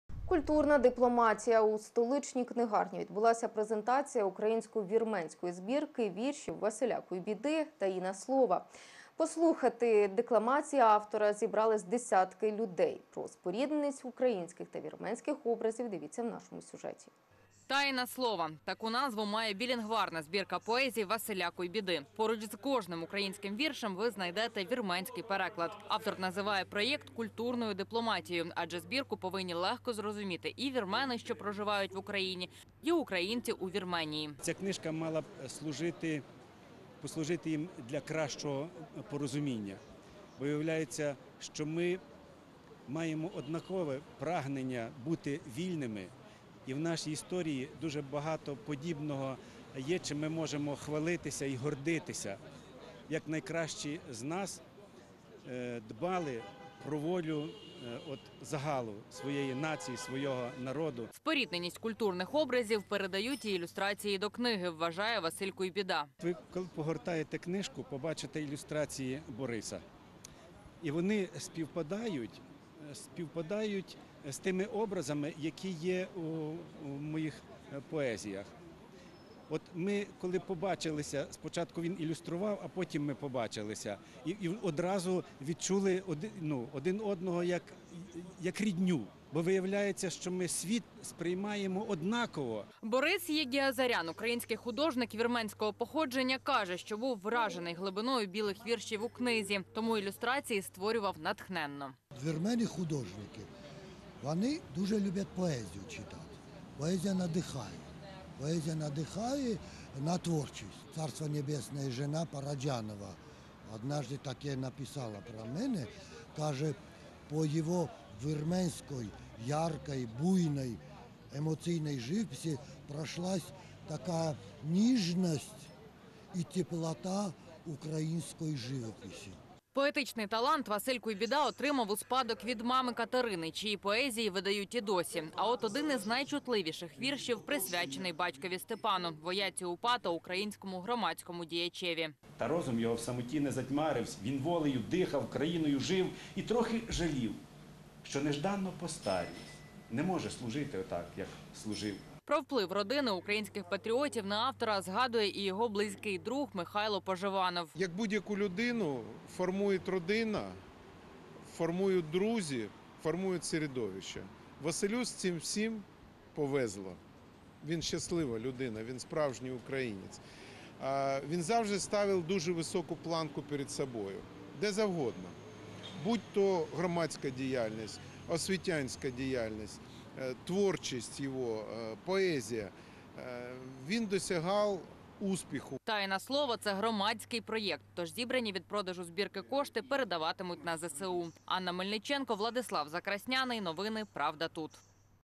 Василь Куйбіда презентував нову поетичну збірку «Таїна слова»
Український громадський діяч Василь Куйбіда презентував у Києві свою нову збірку поезій «Таїна слова». Книга вийшла двома мовами: українською та вірменською. Автор каже, що між нашими культурами є багато спільного, тож хоче аби українці у Вірменії, та вірмени в Україні однаково розуміли його вірші. “Таїна слова” це громадський проєкт, тобто видавництво не фінансувалось на рівні держави ні Україною, ні Вірменією, проте зібрані кошти з продажу автор відправлятиме на підтримку Збройних сил України. Василь Куйбіда видав  вже понад 10 віршованих збірок. Автор ділиться,поетичний талант у нього від мами Катерини Куйбіди, чиї вірші актуальні й досі.